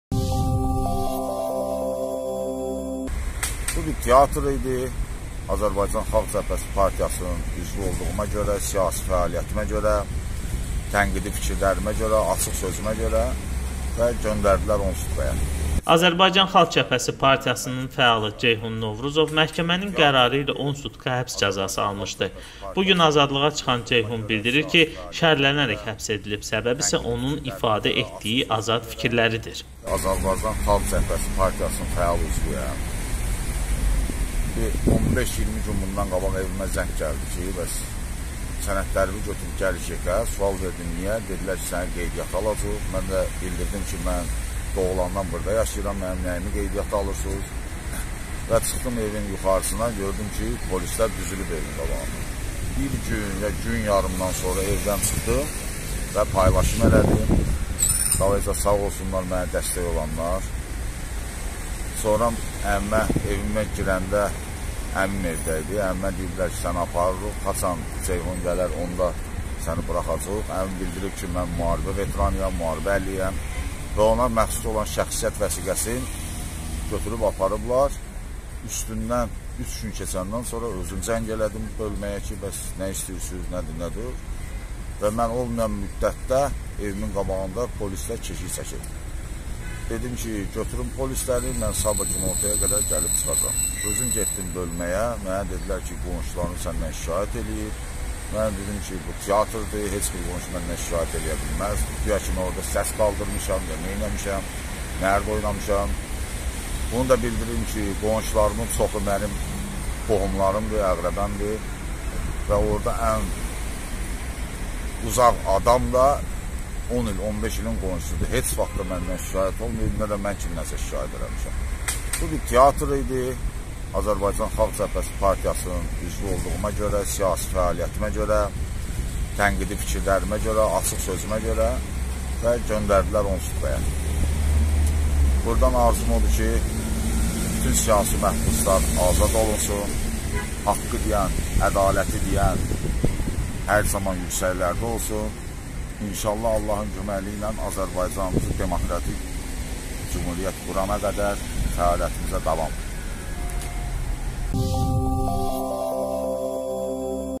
"Məni tənqidi fikirlərimə görə, şərləyərək həbs etdilər, bu bir teatr idi"-AXCP fəalı
Azərbaycan Xalq Cəbhəsi Partiyasının fəalı   Ceyhun Novruzov məhkəmənin qərarı ilə 10 sutka həbs cəzası almışdı.Bu gün azadlığa çıxan Ceyhun bildiri ki,şərlənərək həbs edilib.Buna səbəb isə onun  ifadə etdiyi azad fikirləridir